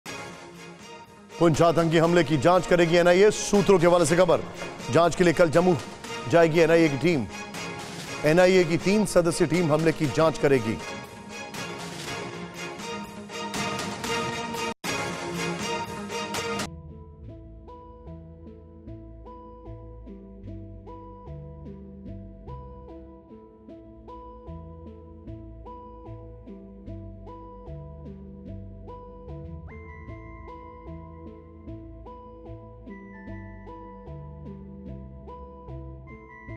Poonch आतंकी हमले की जांच करेगी NIA, जांच के लिए कल Jammu जाएगी NIA की टीम- सूत्र
Poonch terror Attack: पुंछ आतंकी हमले की जांच करेगी NIA, जांच के लिए कल जम्मू जाएगी NIA की टीम, NIA की 3 सदस्यीय टीम हमले की जांच करेगी- सूत्र